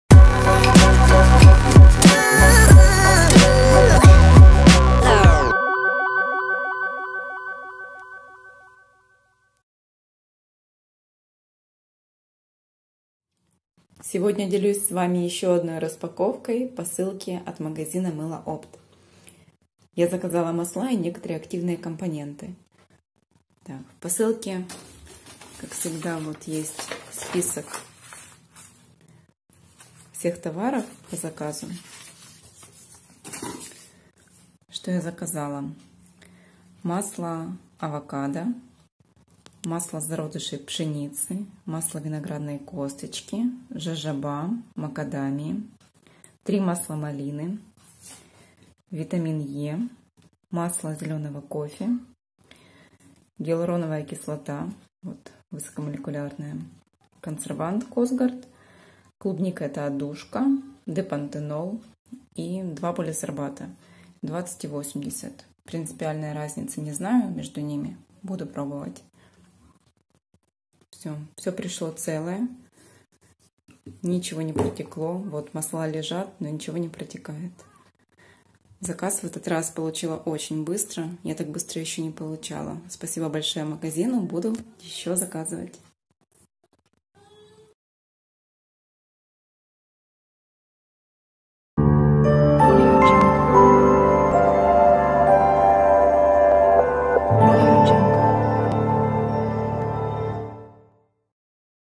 НОВИНКИ от МЫЛО ОПТ уже на КАНАЛЕ
Распаковка №320 от покупателя в интернет магазине Мыло-Опт.
Очень много новых компонентов и блендов различных масел уже на сайте компании мыло-опт.com.ua
Мы всегда рады новым распаковкам и ждем их с нетерпением , что бы показать Вам наши покупатели мыловары и кремовары.